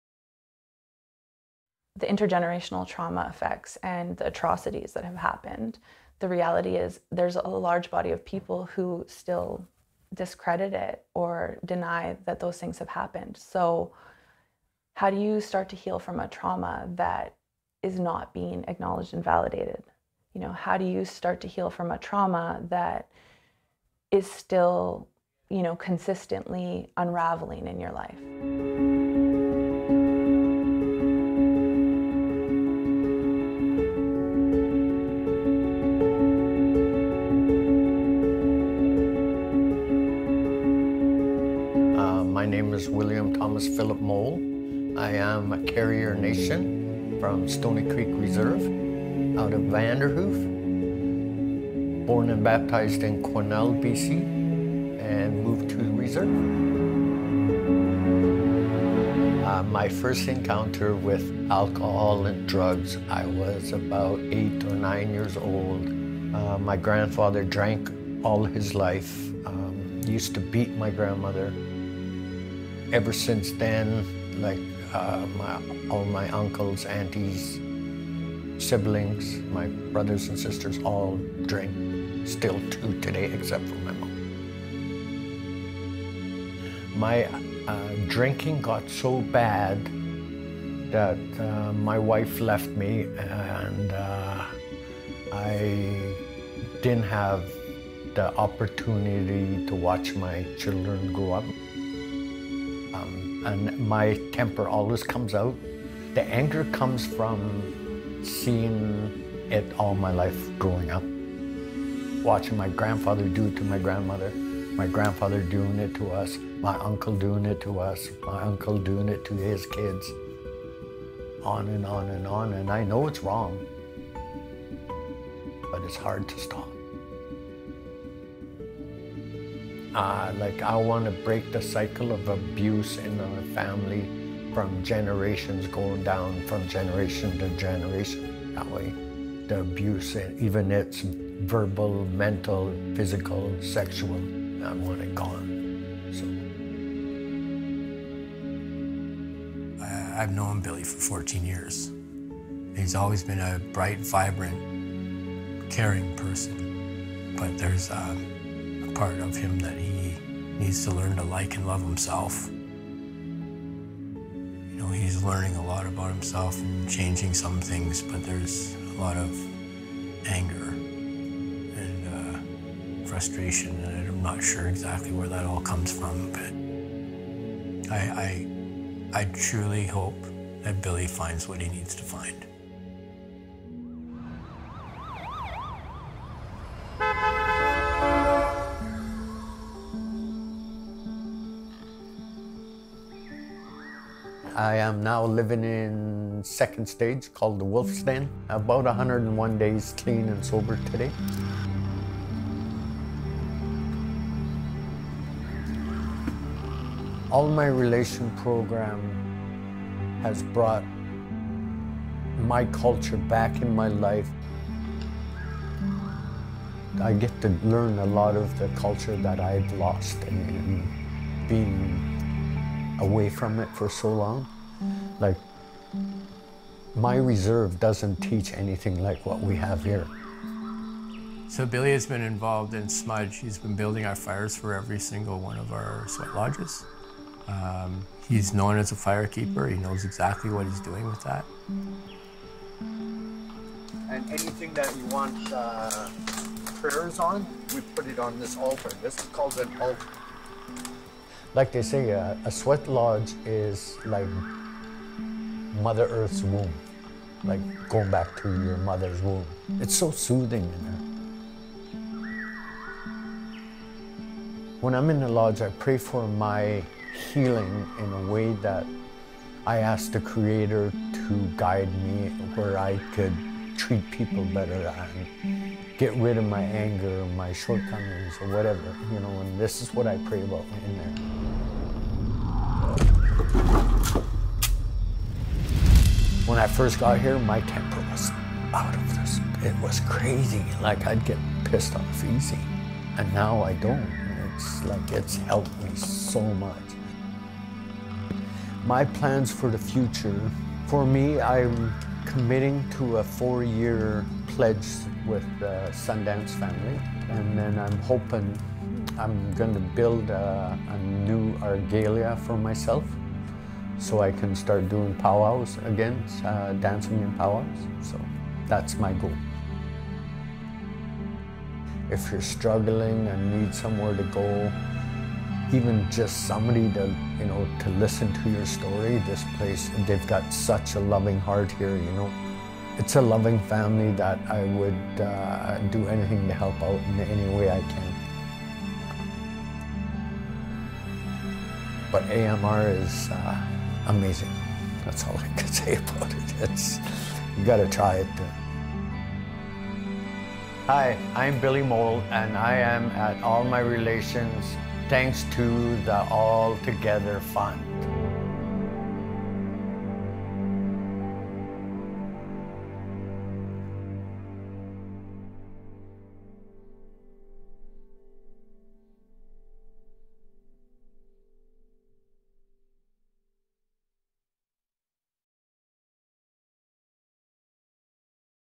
Billy Mole - All Together Fund Story
IN THIS VIDEO:
Every year TWC provides a few select individuals a free treatment stay who would not otherwise be able to access it. See Billy Mole's transformation against adversity and intergenerational trauma as he attends TWC's All My Relations program for indigenous peoples.